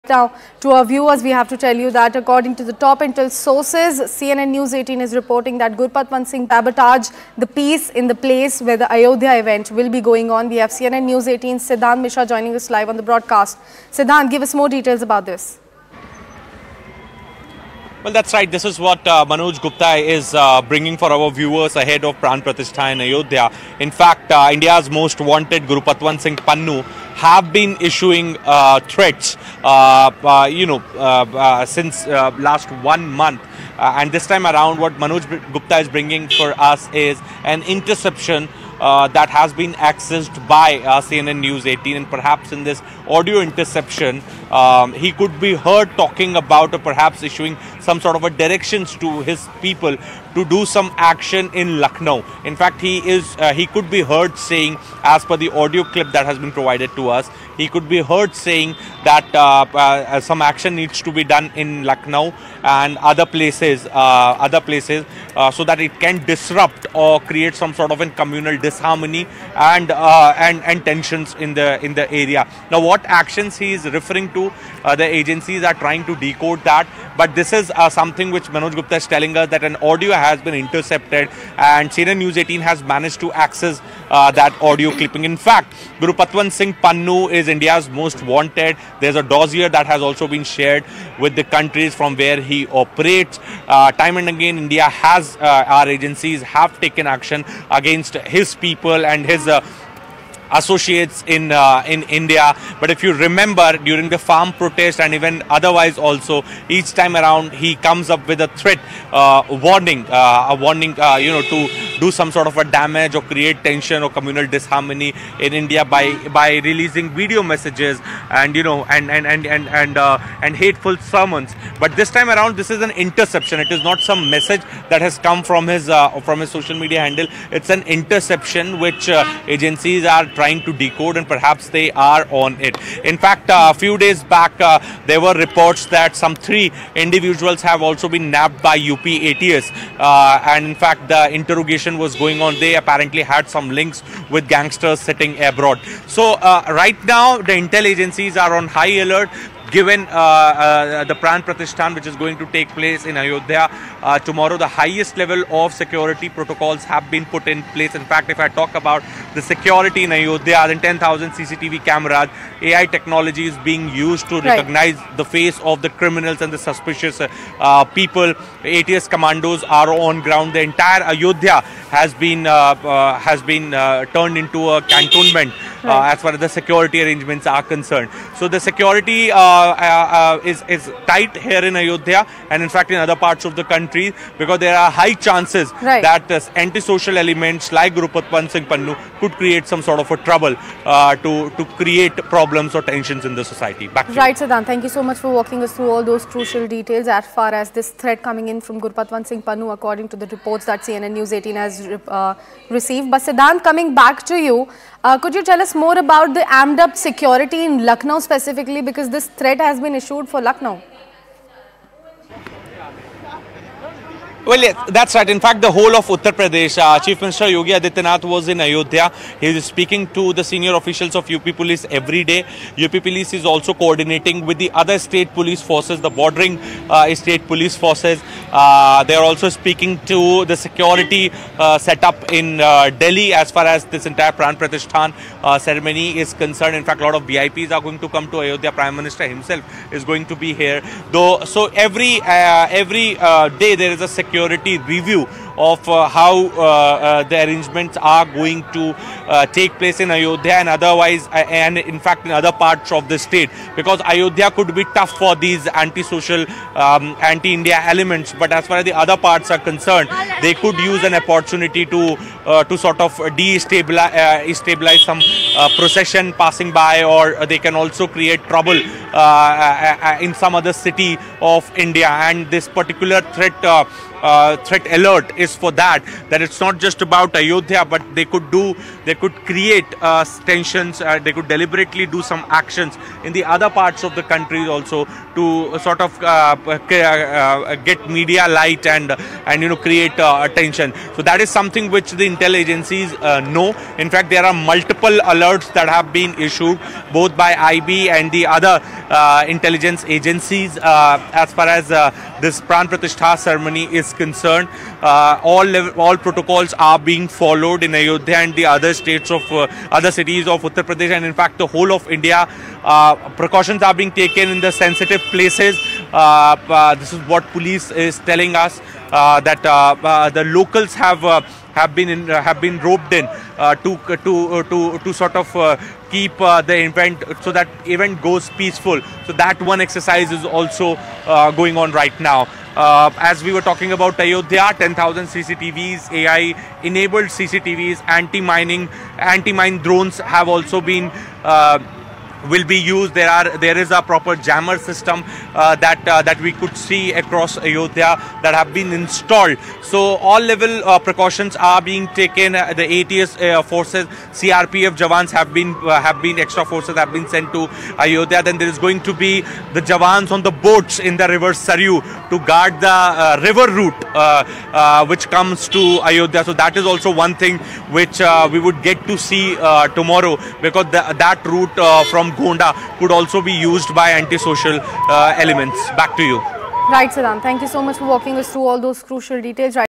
Gurpatwant Singh Pannun Planning Major Attack In Lucknow Ahead Of Ram Mandir Pran Pratishta | News18
Breaking News: Terrorist Gurpatwant Singh Pannun planning major attack in Lucknow ahead of Ram Mandir Pran Pratishta, say top intel sources

n18oc_india